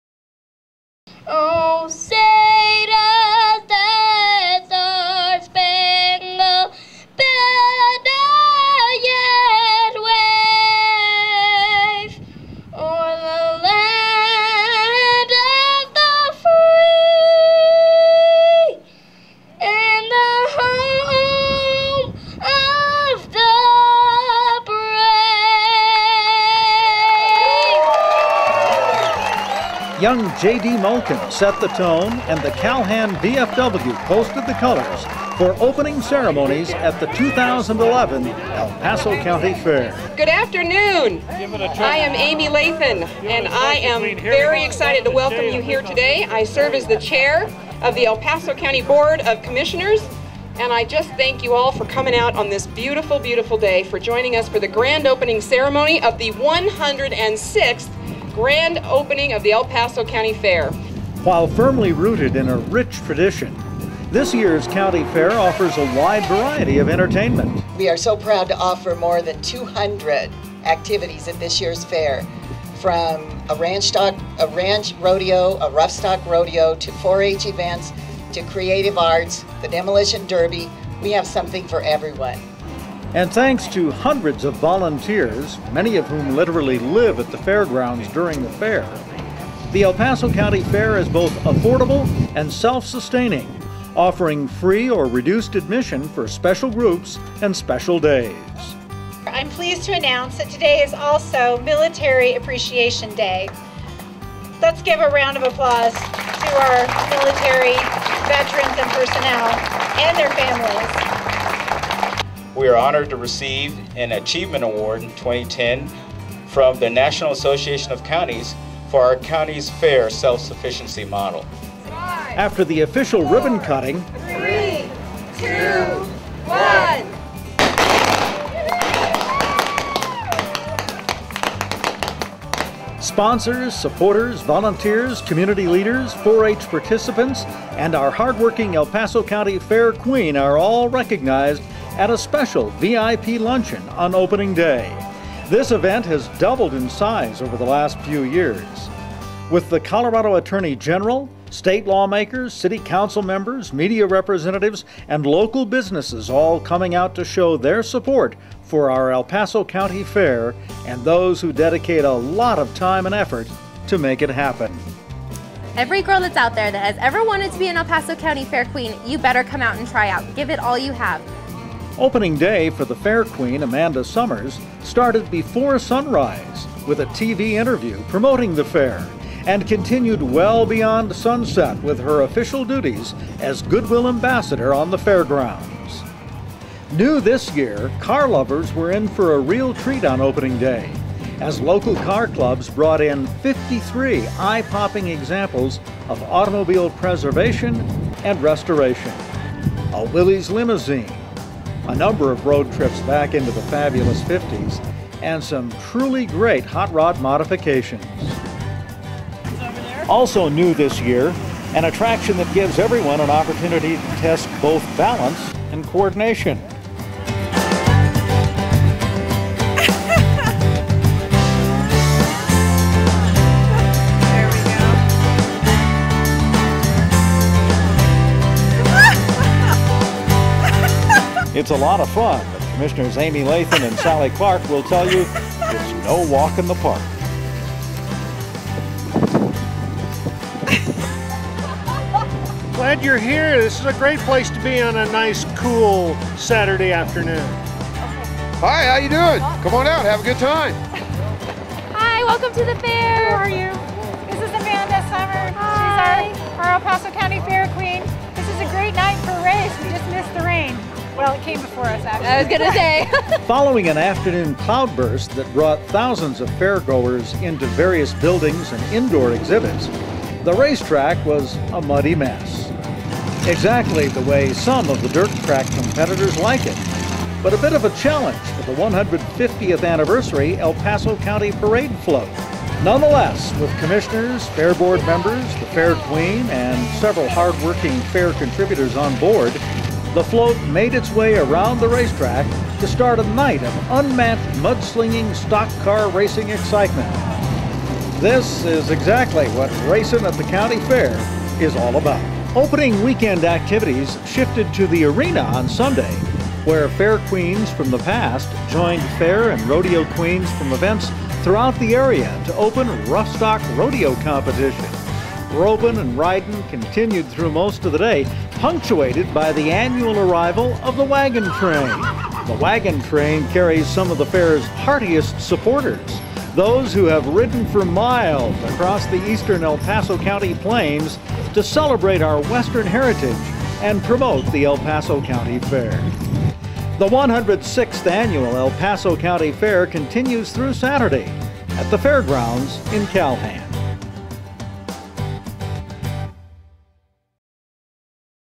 2011 County Fair YT.mov
Check out the good times at the El Paso County Fair in Calhan, Colorado!